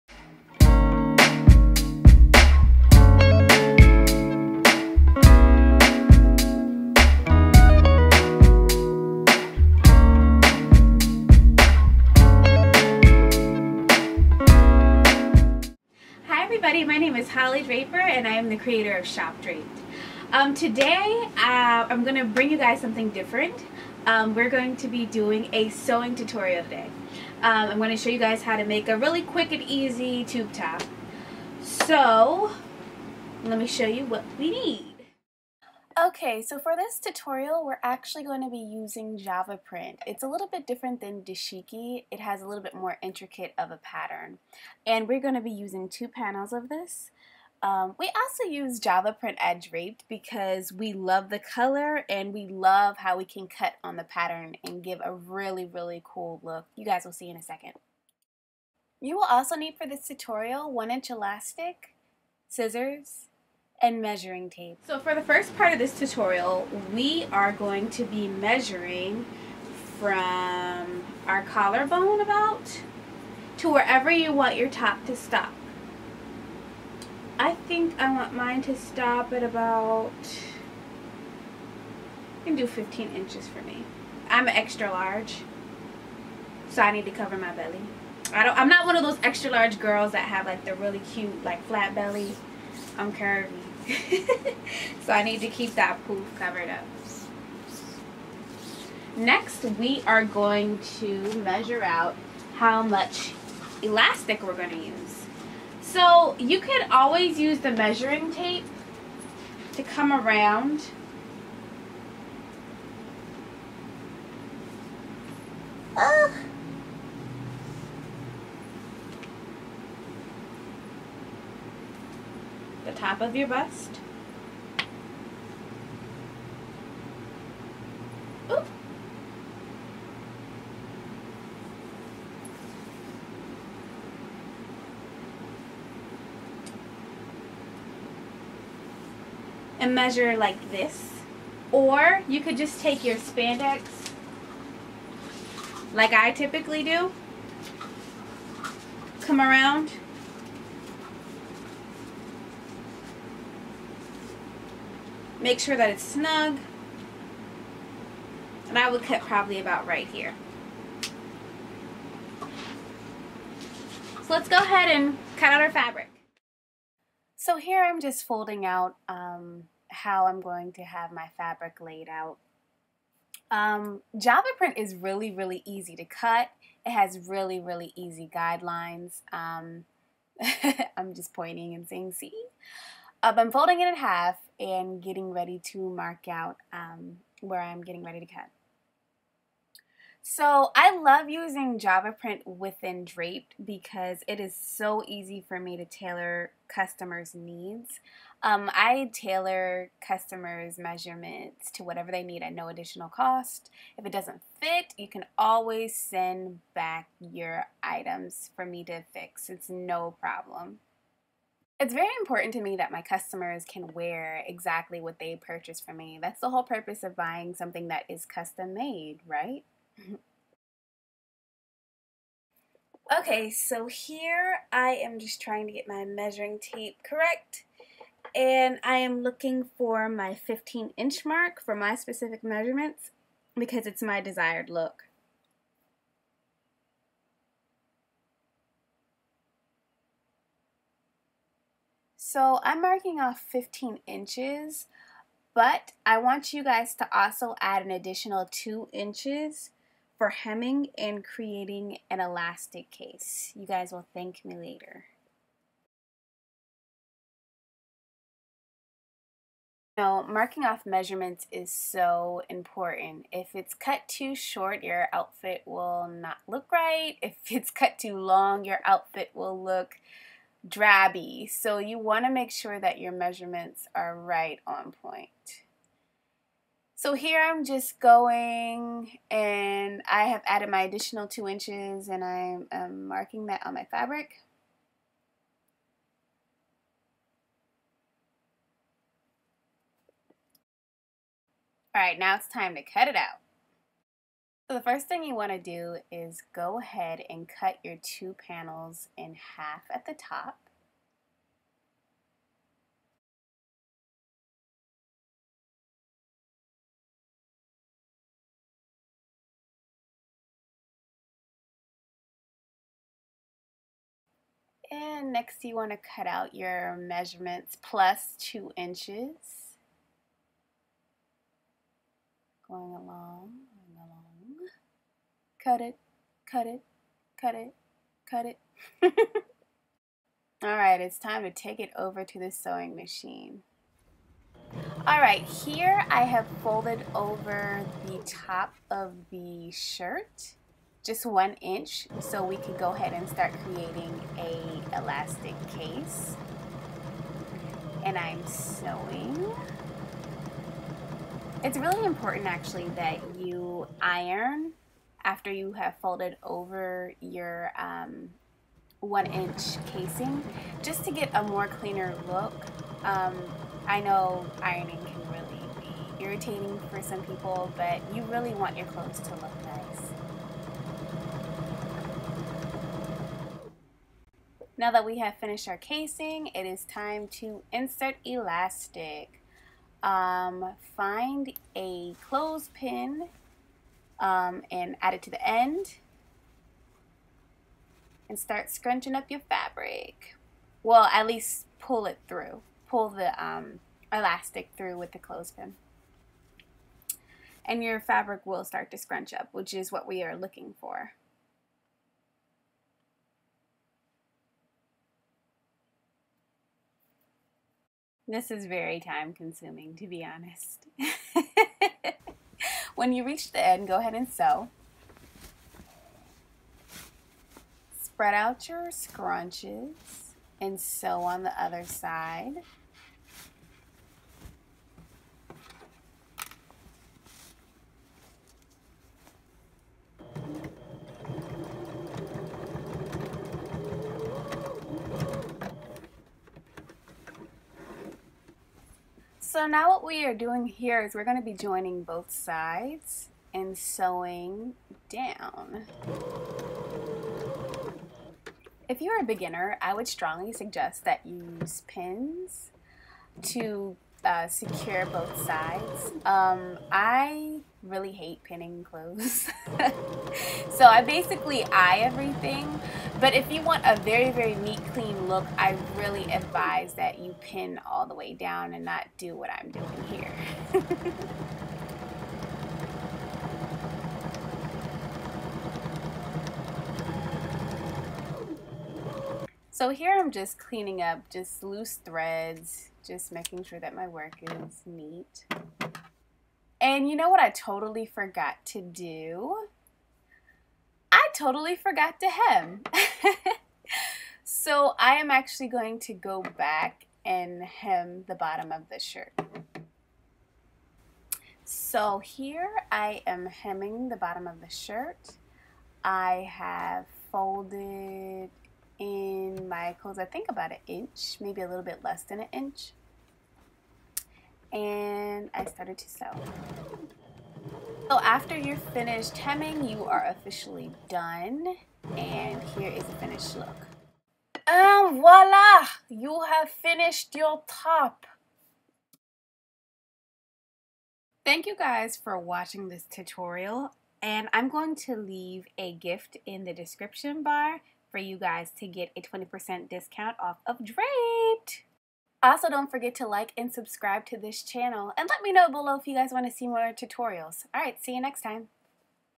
Strapless Top Tutorial
Hey lovelies! In this video I am creating a simple strapless top/tube top. For this tutorial you will need:
A sewing machine
2 Panels of Java Print or Dashiki Print
Elastic
Scissors
Measuring Tape
Fabric Chalk
And a clothespin